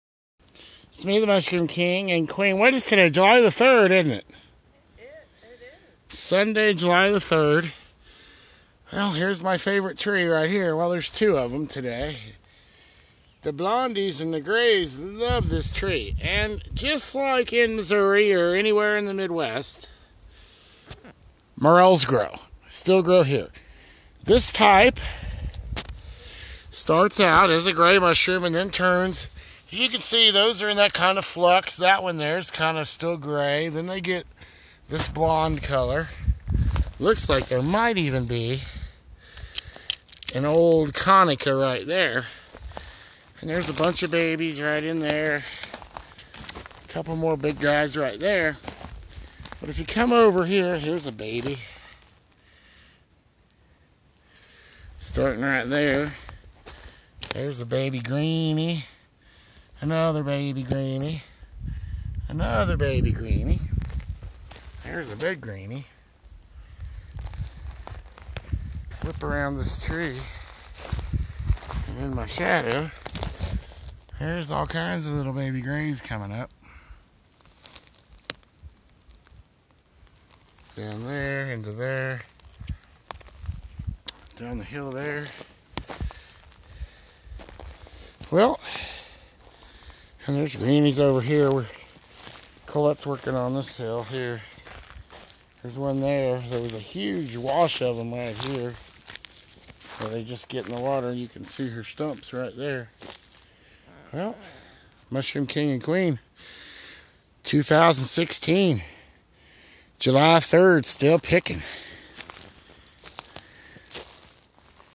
Morel mushroom hunting Farm Patch 1 of 2 July 3rd 2016
This is a spot the Queen and I farmed for weeks.  We would pick the larger or dryer morels. On a five day rotation, which lead to greater yields. The next video is taken on July 8th with much larger and baby morels still.